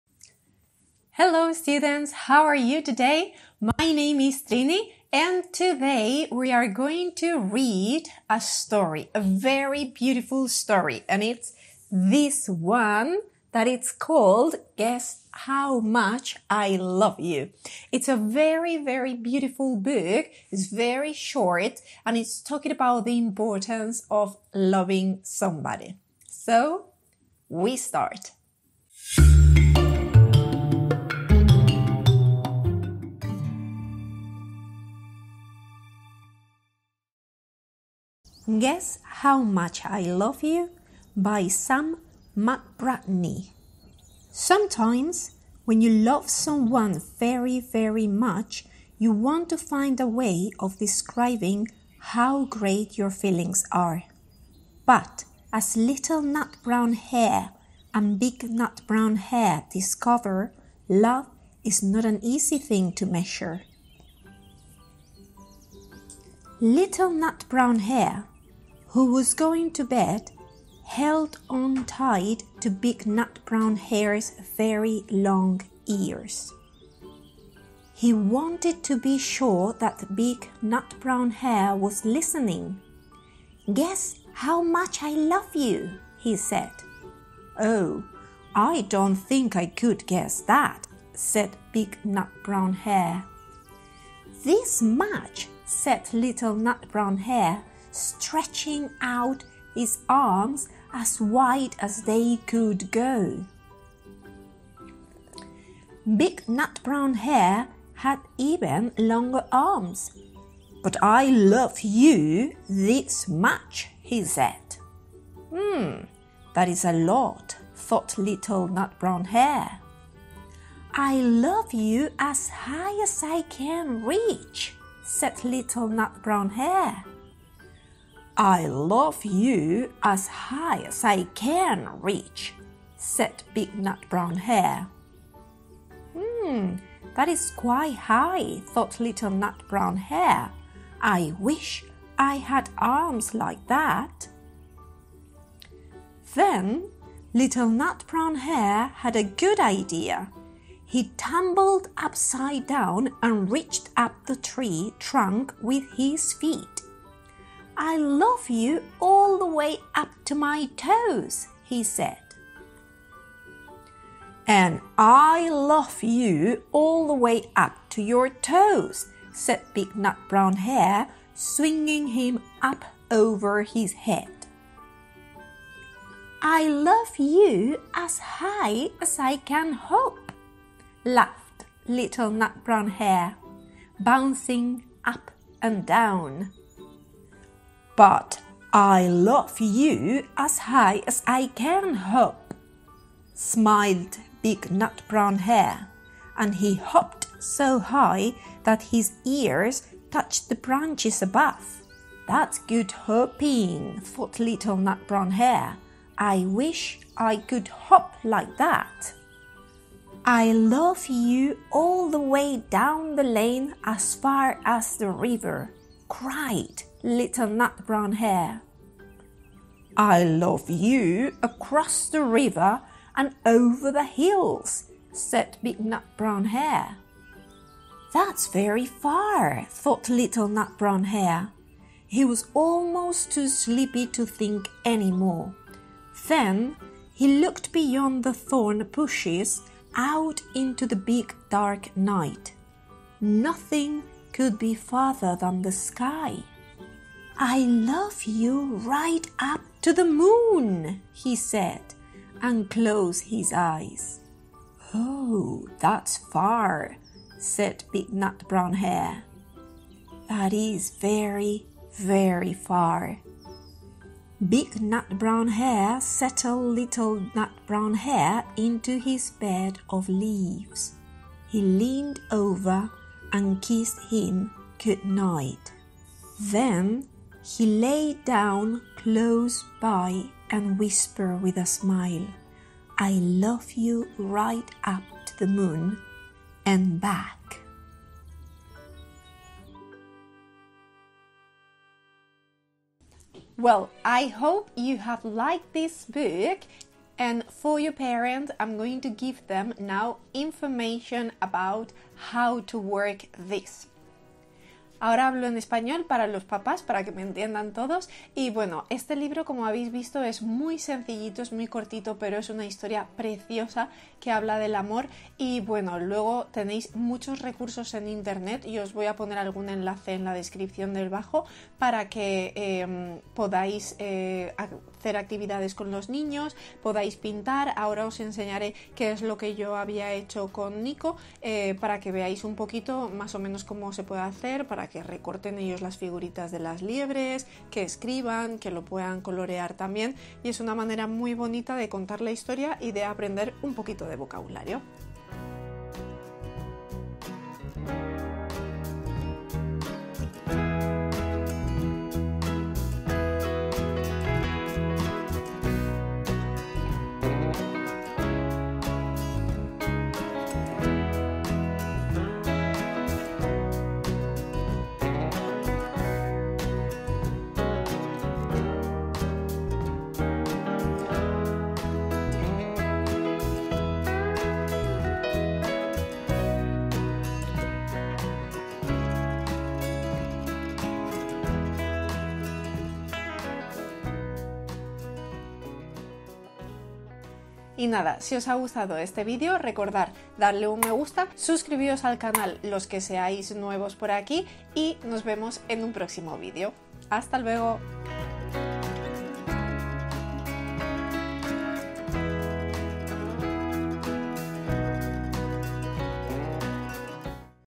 Cuento leído: GUESS HOW MUCH I LOVE YOU (TALE NARRATION) | BRICKFIELD IDIOMAS
Esperamos que os guste esta preciosa historia sobre lo mucho que podemos llegar a querer a alguien gracias a estas dos liebres tan simpáticas.

VEN A VISITARNOS!
Estamos ubicados en:
Avenida Pio XII, 45 entresuelo.
12540 Villarreal, Castellón. (Spain)

PUEDES ENCONTRAR MÁS RECURSOS Y TODA LA INFORMACIÓN DE NUESTROS CURSOS PRESENCIALES Y ONLINE EN NUESTRA WEB: